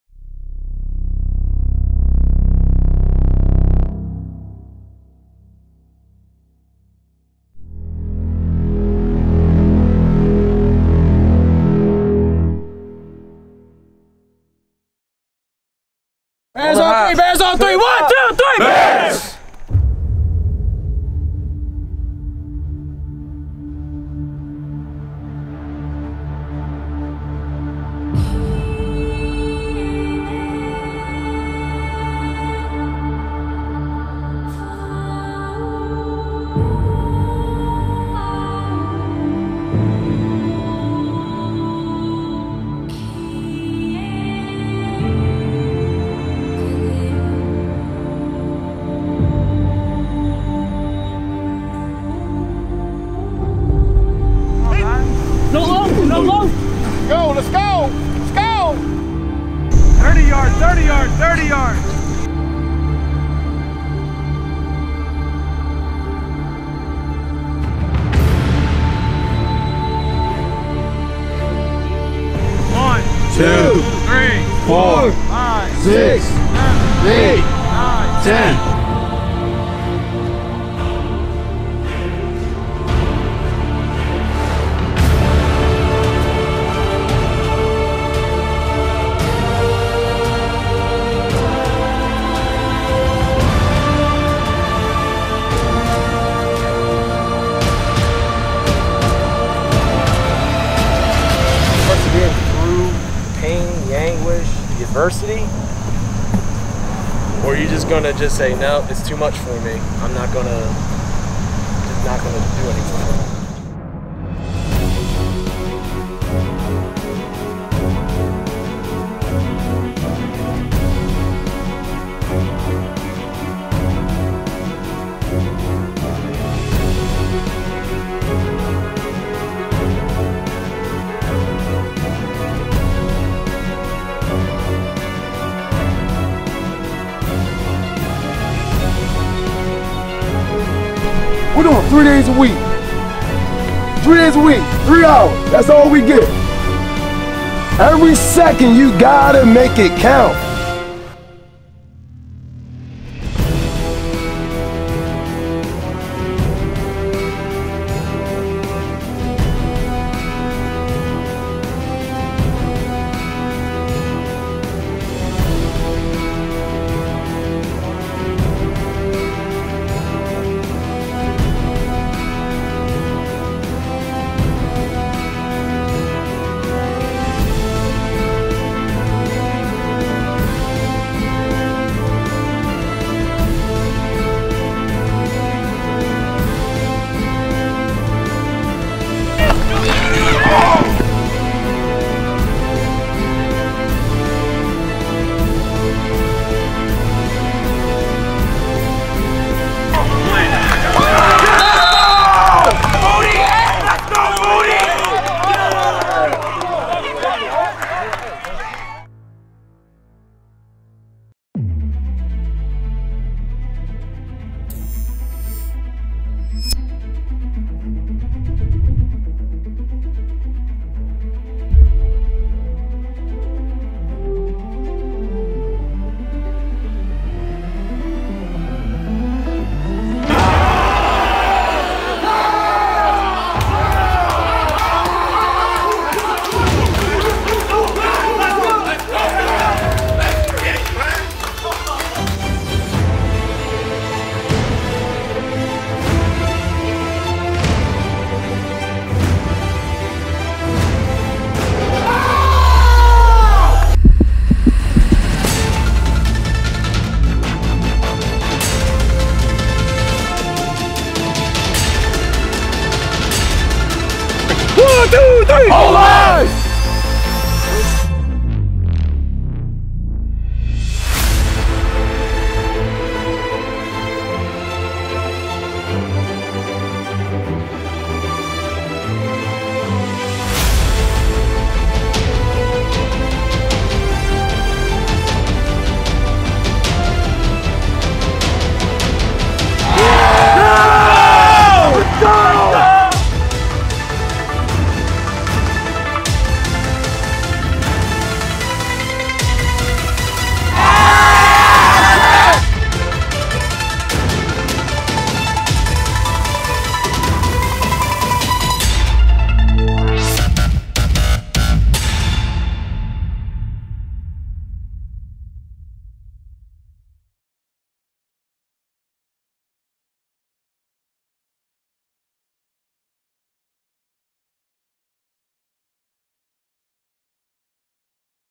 2017 Hammond High Football Promo
Thank you to all the moving parts that allowed us to create this epic video for our favorite local High School. Special thanks to AD Mike Lerner & Head Ball Coach Shawn Frederick. Good luck this year and enjoy our promo video for the 2017 Football Season!